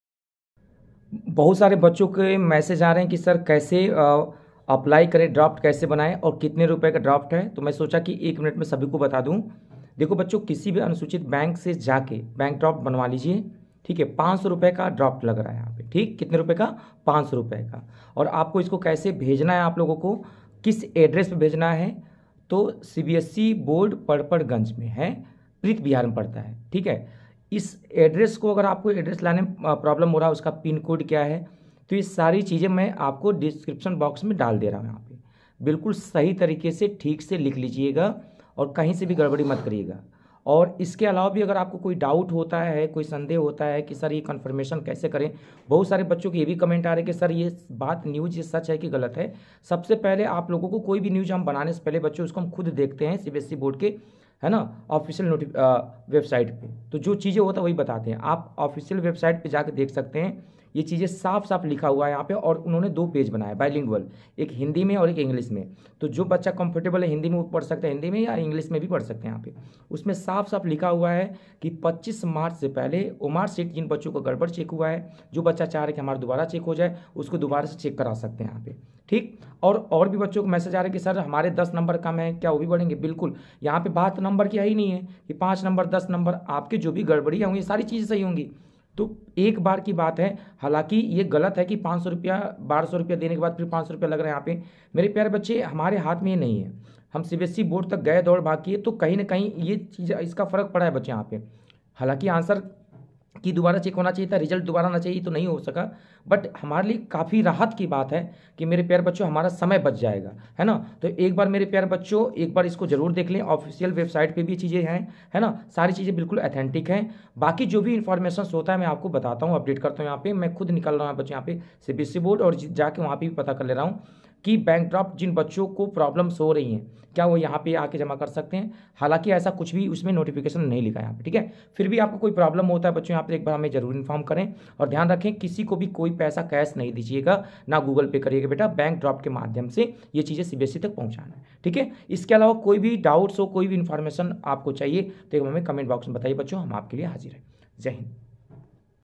CBSE :- CTET Fail Students कैसे करे Apply Bank Draft किस बैंक से बनवाके भेजना है जाने ??
CBSE Board. Address:-   Central Board of secondary education,P S-1-2, Institutional Area , IP Extn, Patparganj Delhi -110092

           Call  For Any Query :-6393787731